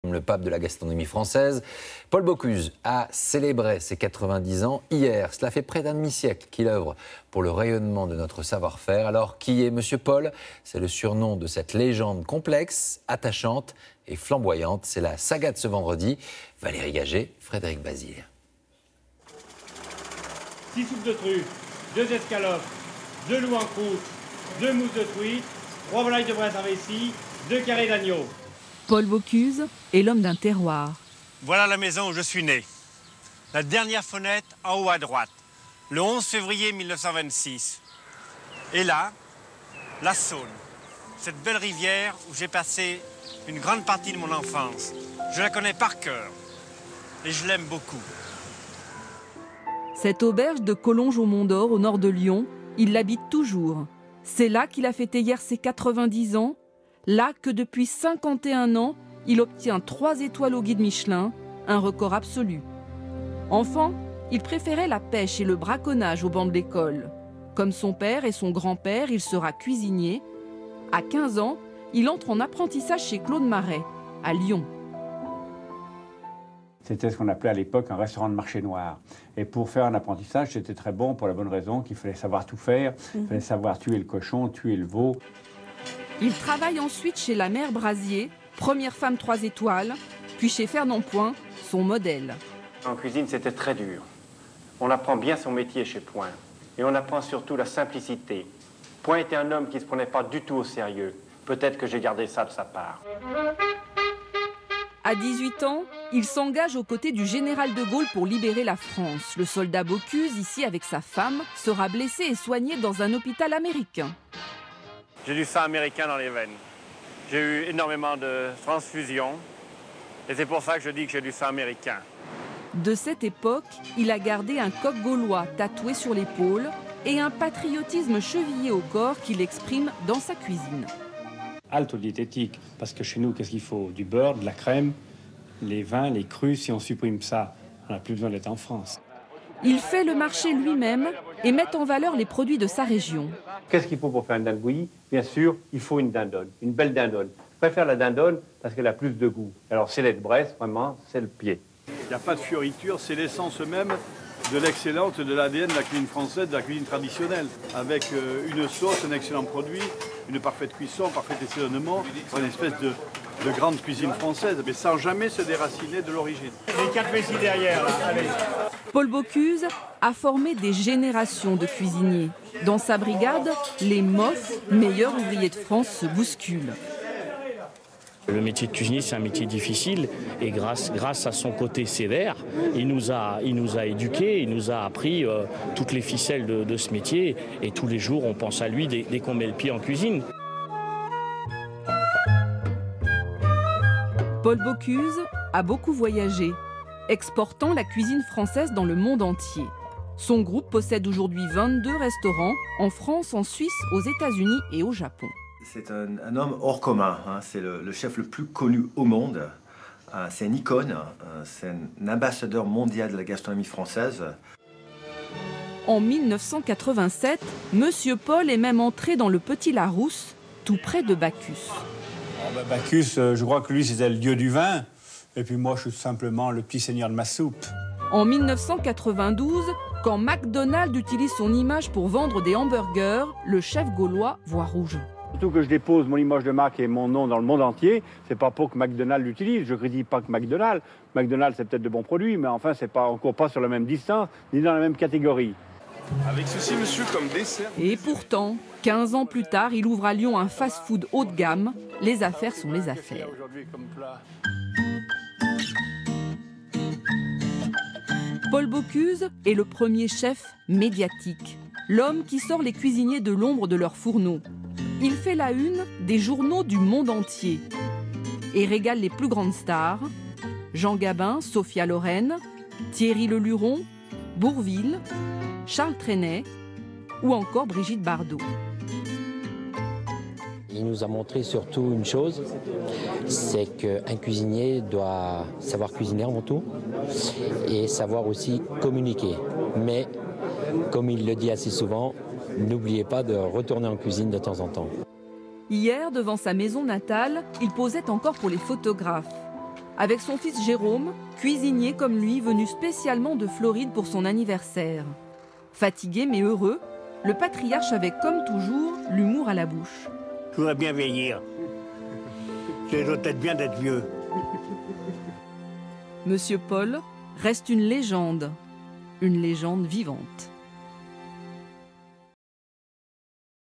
90 ans de Paul Bocuse, 2016
Reportage France 2 sur la vie et le parcours de Paul Bocuse, chef cuisinier 3 étoiles de Collonges.